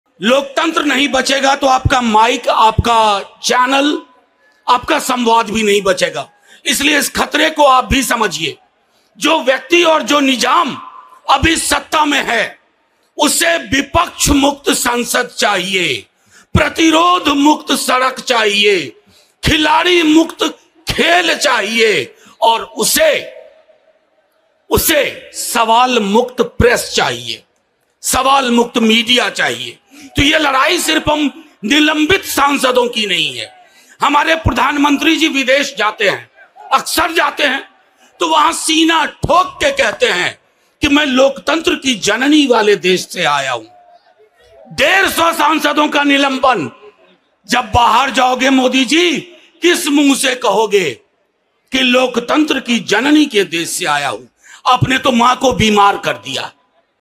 "They want sawal-mukt media" | INDIA Alliance Protest | Manoj Jha
INDIA leaders protest against suspension of 143 INDIA parties MPs at Jantar Mantar, New Delhi